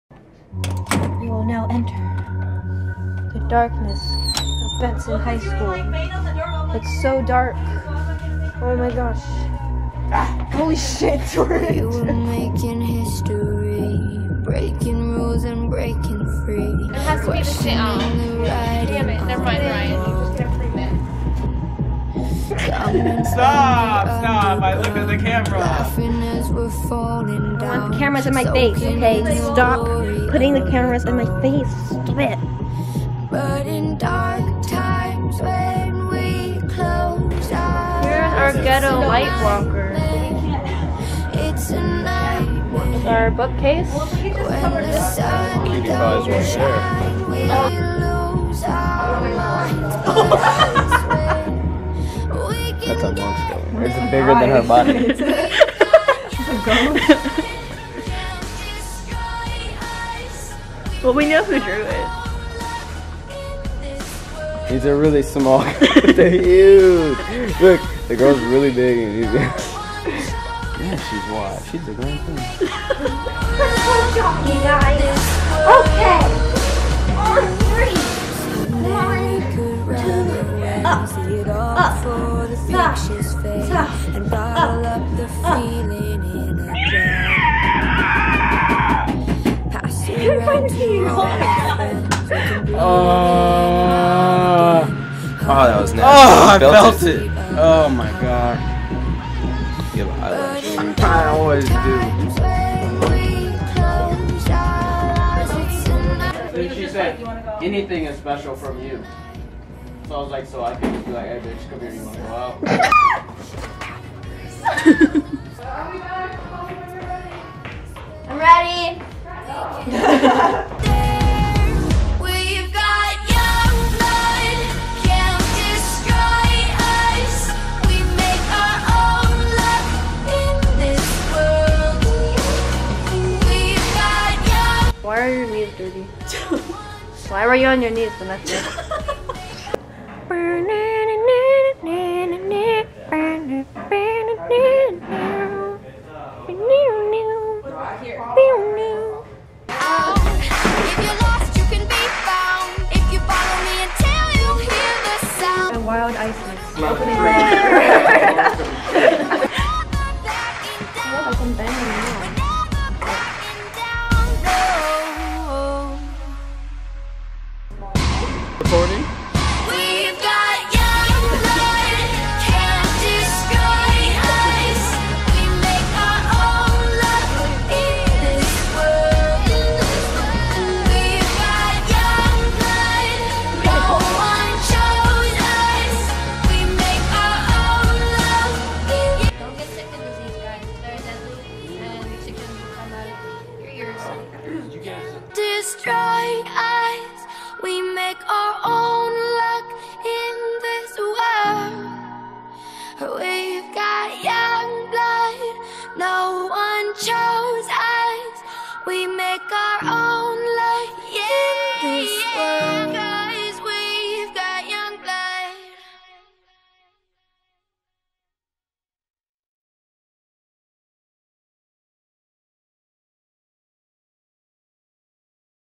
Junior Year Film- Benson Polytechnic High 2014
A little something I put together. It just summed up all the stuff we did for video so I thought why not.